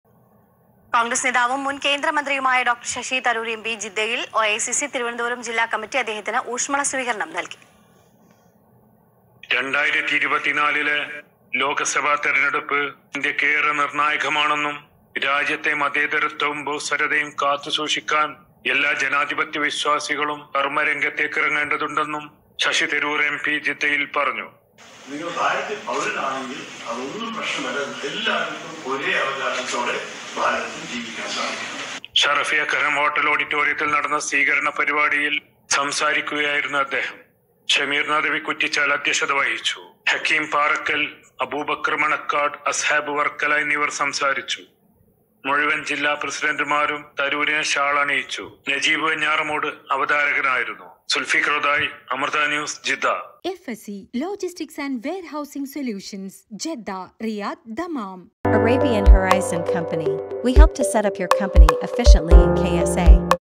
DR ശശി തരൂർ എം.പിക്ക് ജിദ്ദയിൽ സ്വീകരണം. ലോക്‌സഭ തെരഞ്ഞെടുപ്പ് - ഇന്ത്യക്ക് ഏറെ നിർണായകം.
കോൺഗ്രസ് നേതാവും മുൻ കേന്ദ്ര മന്ത്രിയുമായ DR ശശി തരൂർ എം.പിക്ക്  ജിദ്ദയിൽ ഊഷ്മള സ്വീകരണം
2024-ലെ ലോക്‌സഭ തെരഞ്ഞെടുപ്പ് - ഇന്ത്യക്ക് ഏറെ നിർണായകം. സുൽഫീക്കർ ഒതായി, അമൃത ന്യൂസ്